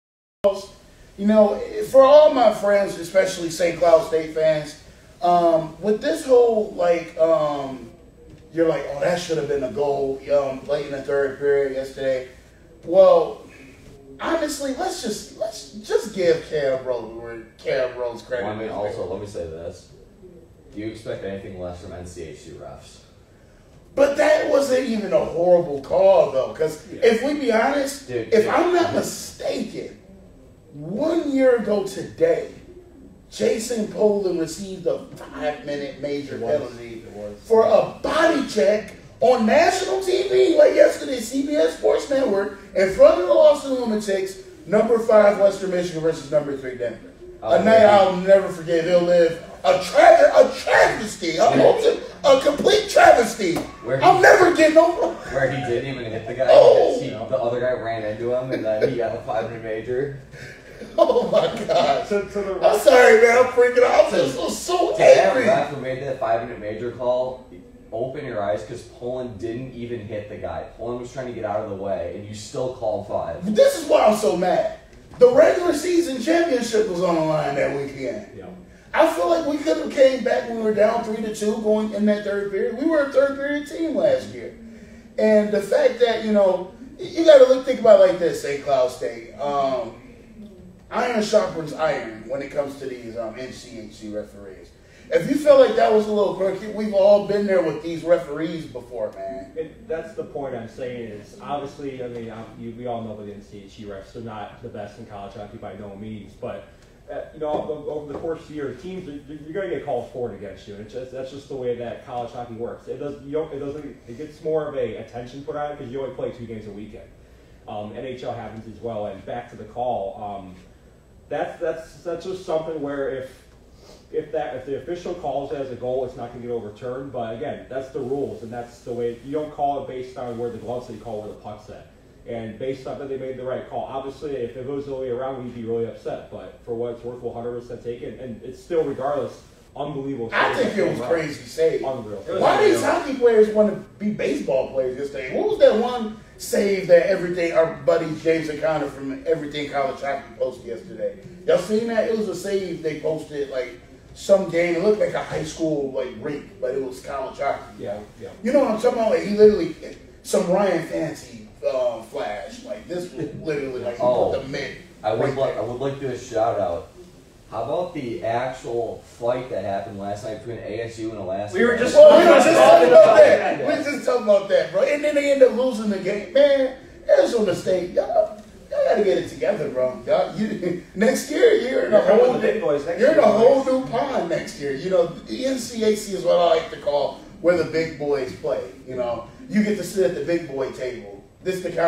Wheeler Takes: #10 WMU vs #11 SCSU-Did Cam Rowe save the puck? Was it a good goal?
Welcome back to the channel and another episode of Wheeler Takes in your host Chris Wheeler

Here on this edition of Wheeler Takes me and 4 of my special guests discuss one of the bigger sports alongside football here at Western Michigan University and that is Division-1 College Hockey..

Last weekend the 10th nationally ranked Western Michigan Broncos took on their conference rival 11th nationally ranked St. Cloud State Huskies in a battle of two of the top-15 teams in the nation on CBS Sports network at Herb Brooks Ice Arena

So much was at stake in this pivotal series: Home ice for the conference playoffs, ncaa rankings, and a potential national collegiate hockey conference (NCHC) Semi-Finals rematch…

The huskies started off the Friday night game up 3-1 on the broncos going into the 3rd period…but WMU’s  high-powered offense soared back into this game to tie the game 3-3. Then one of the SCSU players shot what appeared to be a gamewinning goal with 1:38 left in the game

However, the WMU goalie Cam Rowe made a save that prevented the puck from cross the goal net line..The referees reviewed it to see if the puck cross the line therefore the huskies didn’t get that goal to count

WMU went on to win in overtime 4-3 thanks to a gamewinning goal from freshman sensation Alex Bump and SCSU won the Saturday game 3-0  to split the weekend series

The question remains….Was it a good goal? Or did Cameron Rowe just make a amazing highlight reel save?

Listen to the discussion with me and fellow WMU fans on an episode of wheeler takes…

Wheeler Takes, LIVE Mondays at 2:00 pm, Fridays at 10:00 am, and Saturdays at 11:00 am on the WIDR App🎥🎬